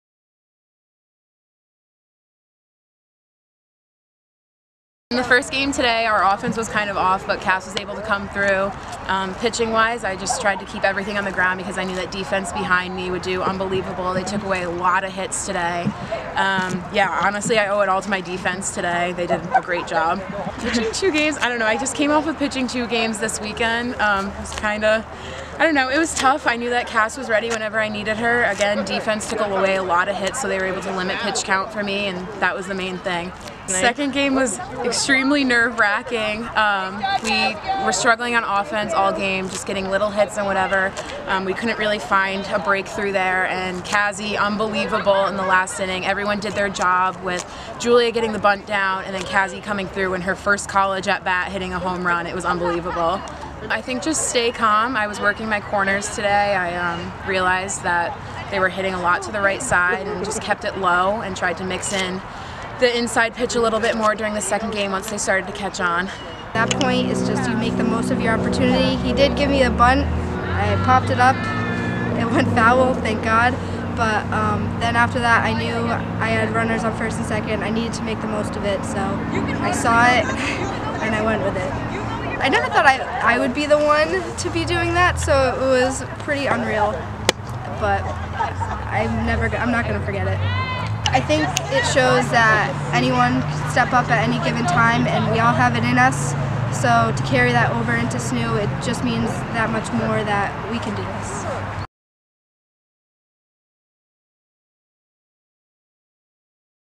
Softball Sweeps Bentley with Walk-Off Homeruns
The Stonehill softball team swept Bentley University on Wednesday afternoon and Jennifer Mayette earned two wins and two shutouts as Kazi Walker recorded her first-collegiate homerun, hit, and walkoff.